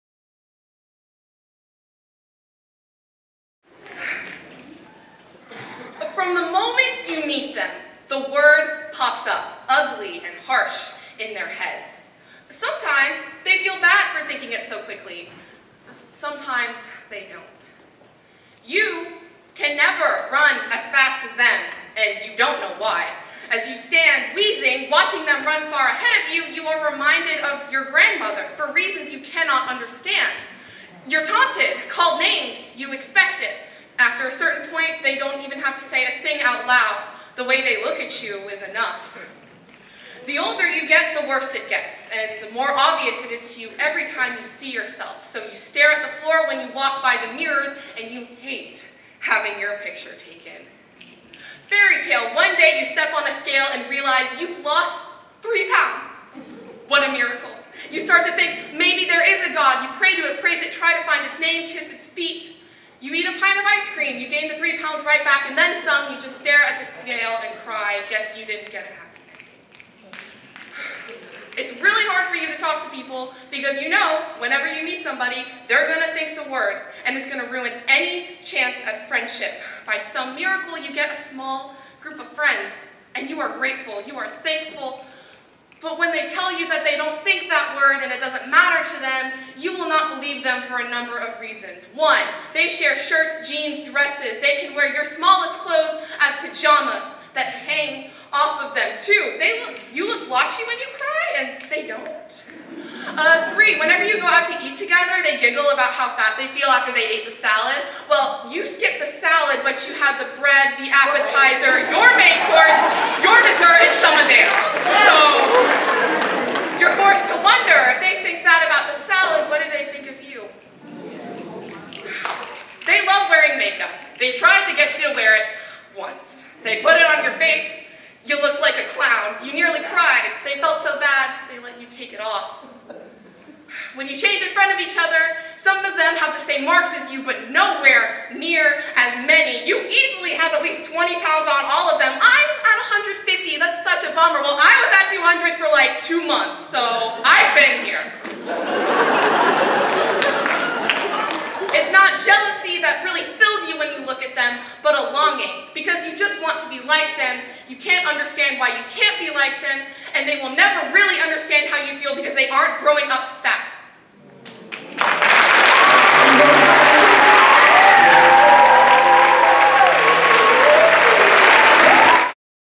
Skye - State of the Word - Southern Word
The State of the Word is the region's leading youth poetry event, which brings together college poets with their high school counterparts from Davidson, Rutherford, Williamson, Sumner, Shelby Counties and beyond at Vanderbilt University's Blair School of Music. Southern Word places writer mentors in schools, universities, and community organizations.